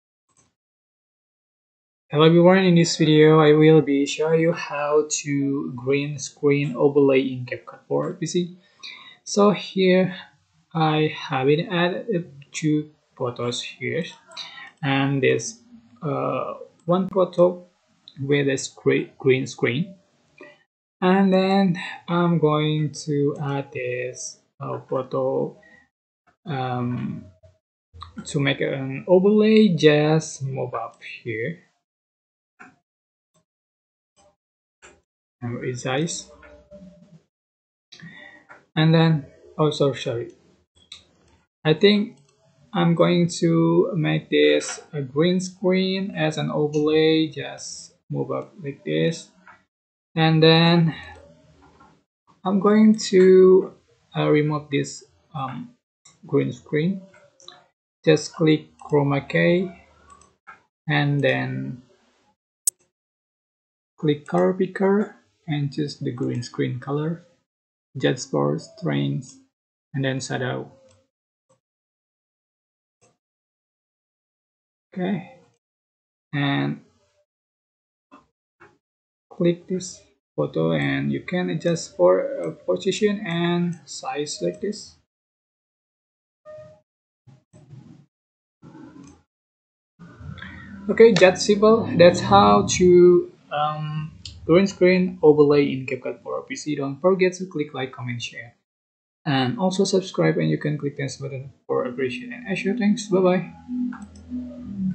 Make A Green Screen Overlay In CapCut For PC Tutorial 2022
This tutorial will show you How To Make A Green Screen Overlay In CapCut For PC.